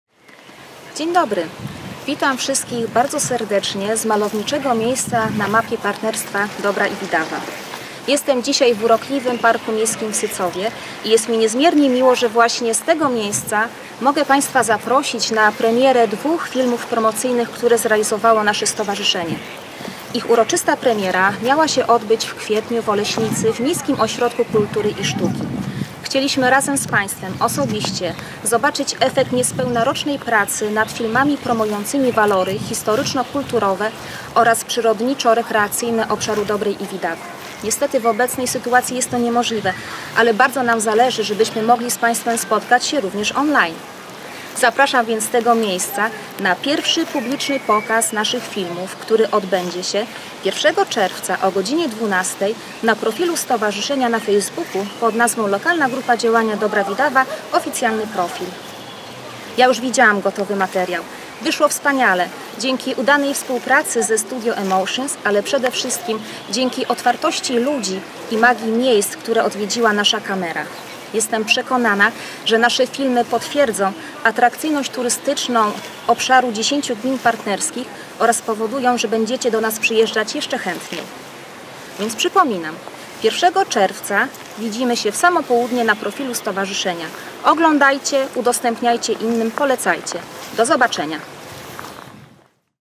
Premiera online - 01.06.2020 r. o godz.12:00
Prezes Stowarzyszenia p. Agnieszka Kasina zaprasza na premierę online filmów promujących walory historyczno-kulturowe oraz przyrodniczo-rekreacyjne obszaru Dobrej Widawy. Premiera odbędzie się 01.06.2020 r. o godz. 12:00 na naszym profilu FB. Zapraszamy również na kanał youtube LGD Dobra Widawa, gdzie możemy znaleźć dwa spoty promocyjne :) .

Realizacja video: Studio Emotions.